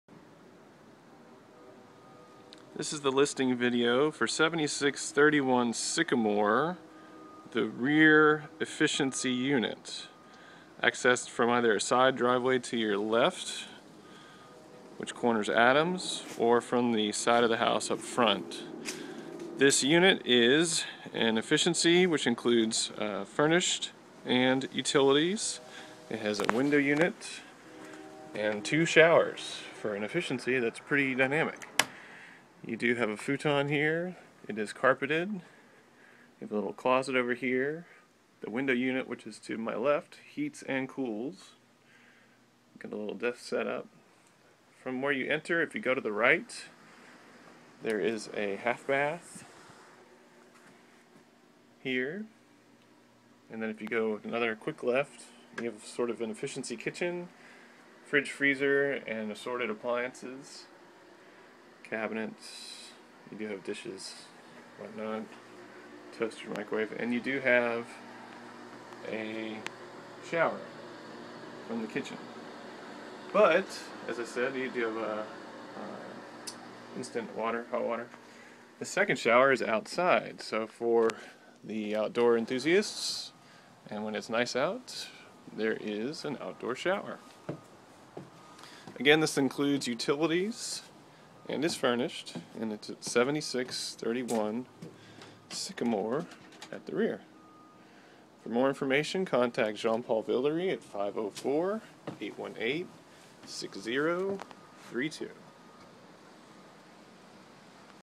7631 Sycamore REAR lease $860
Furnished w/utils Uptown efficiency, no pets, no smoking